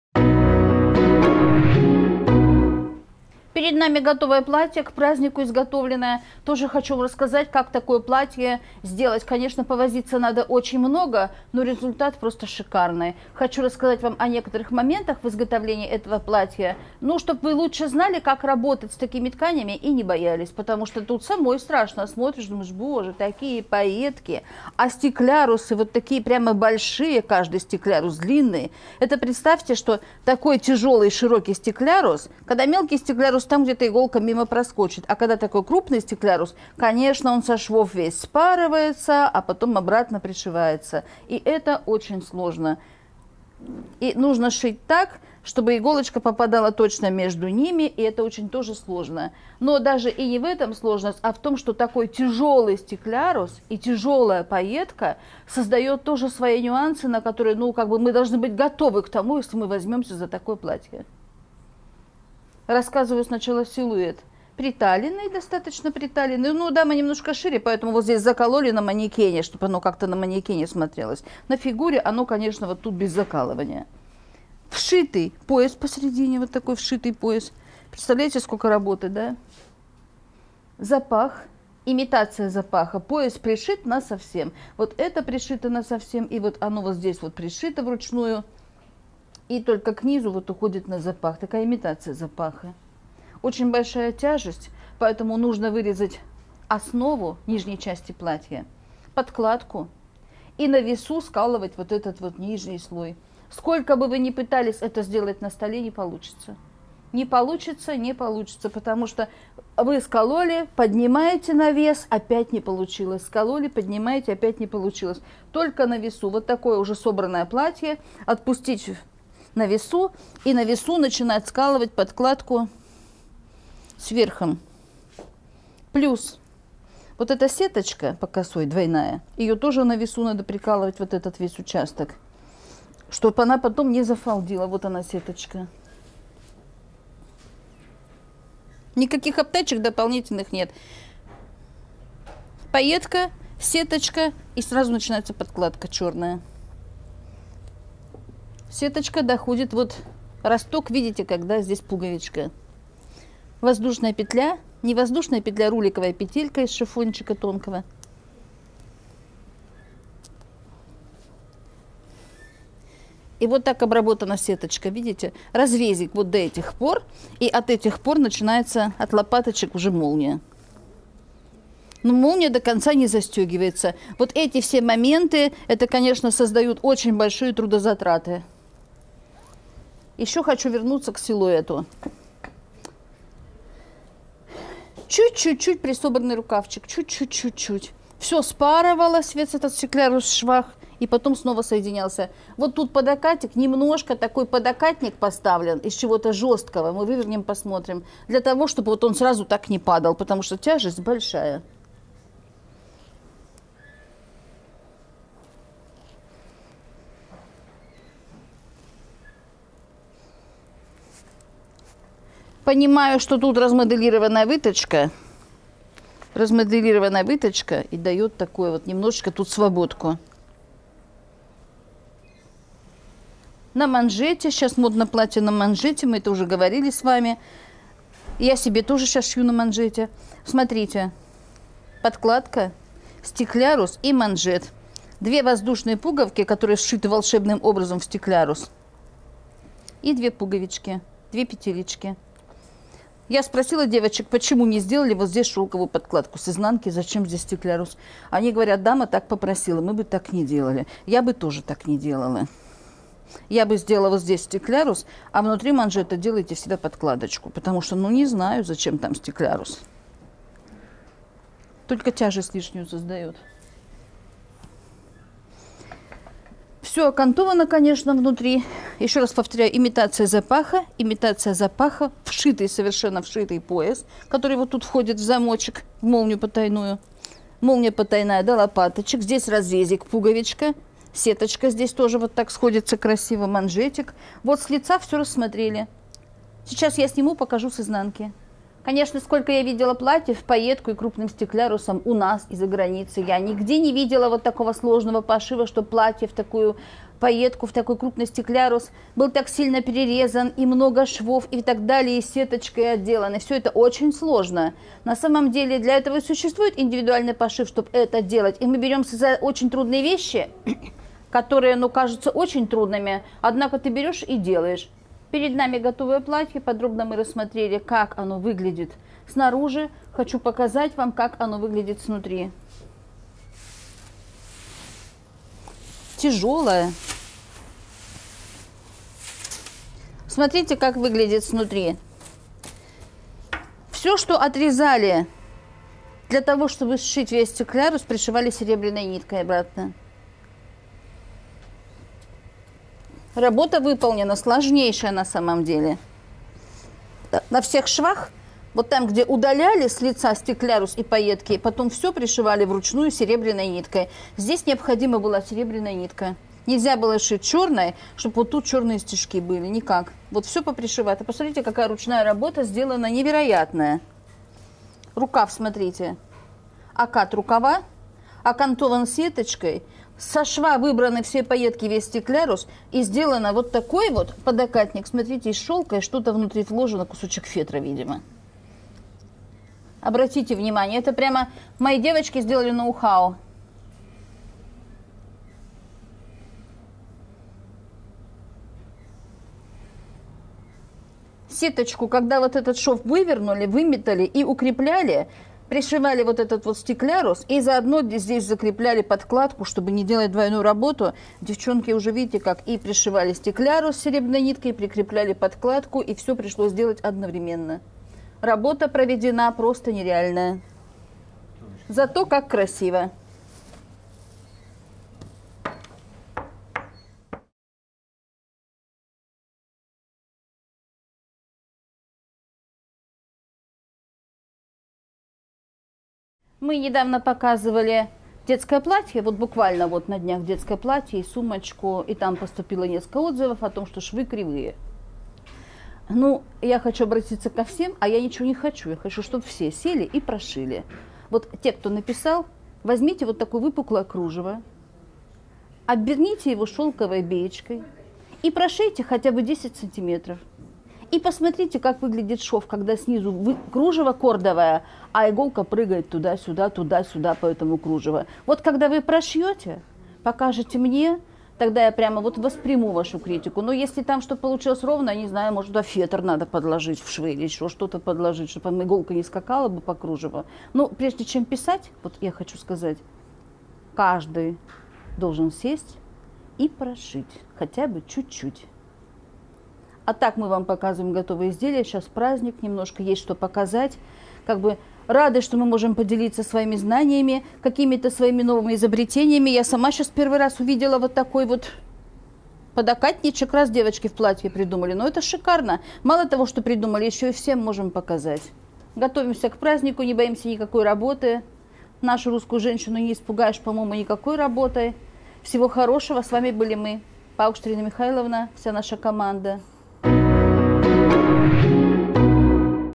Показ готового изделия. Платье с запахом. Как работать с пайеточной тканью с крупным стеклярусом?
📀 Платье футляр на подкладке с рукавом и без рукава. Моделирование и пошив